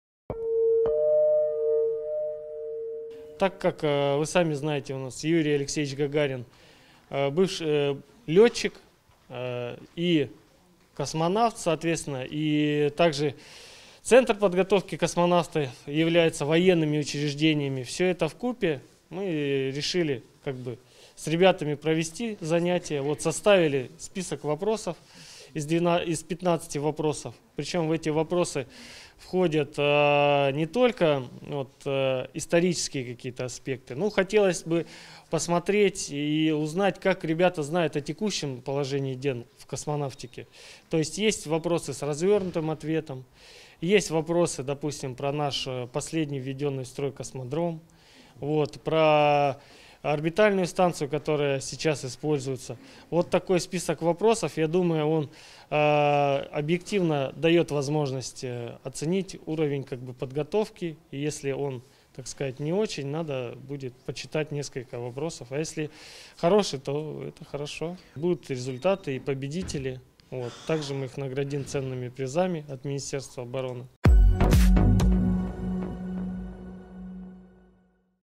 Студенты техникума индустрии питания, коммерции и сервиса написали космический диктант
Космический диктант написали студенты Тюменского техникума индустрии питания, коммерции и сервиса 12 апреля.

Как рассказал один из организаторов, начальник пункта отбора но военную службу по контракту Тюменской области Иван Текучев, Юрий Гагарин был военным летчиком, Центр подготовки космонавтов находился в ведении Министерства обороны, поэтому мероприятие в День космонавтики дополнило работу по патриотическому воспитанию, которую военный комиссариат регулярно проводит с колледжами и вузами региона.

В диктанте - 15 вопросов, среди которых не только исторические факты из прошлого, но и задания на тему текущего положения дел в космической отрасли России. Например, есть вопросы про последний введенный в строй космодром, про Международную космическую станцию и ответить на них нужно развернуто, а не просто выбрать вариант из предложенных.

Иван Текучев считает, что подобранные ими вопросы дают возможность оценить уровень знаний студентов по теме. Итоги подведут сегодня, 12 апреля, лучшие получат призы с символикой Минобороны.

Студентки колледжа, Лилит Саркисян и Светлана Старченко, поделились впечатлениями о диктанте. Свой результат предварительно оценивают как хороший, а от мероприятия получили только положительные впечатления. Девушки уверены, что исследовать космос современному человеку необходимо, нельзя сосредотачиваться только на земных проблемах, ведь Вселенная изучена недостаточно и впереди - множество открытий.